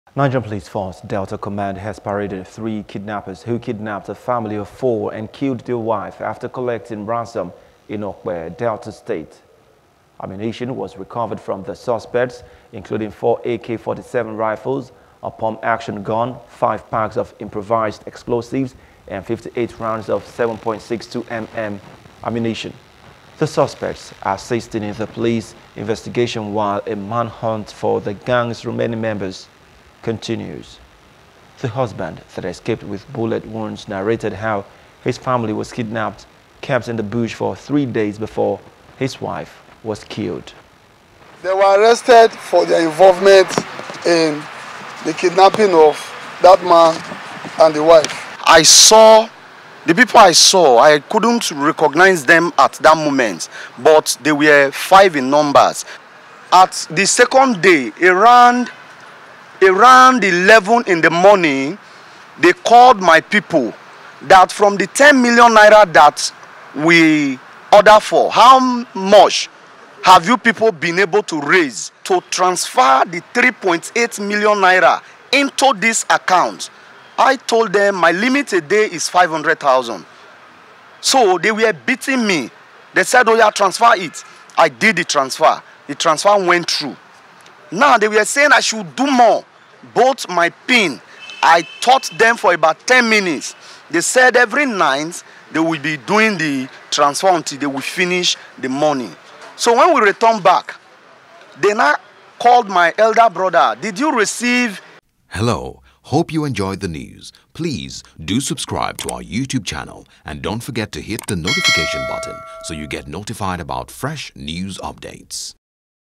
Police Parade Suspects Involved In Murder, Kidnapping Family, After Collecting Ransom
Nigerian Police Force, Delta Command has paraded three kidnappers who kidnapped a family of four and killed their wife after collecting ransom in Opke, Delta State. Ammunition was recovered from the suspects, including four AK-47 rifles, a pump action gun, five packs of improvised explosives, and 58 rounds of 7.62mm ammunition. The suspects are assisting in the police investigation, while a manhunt for the gang's remaining members continues.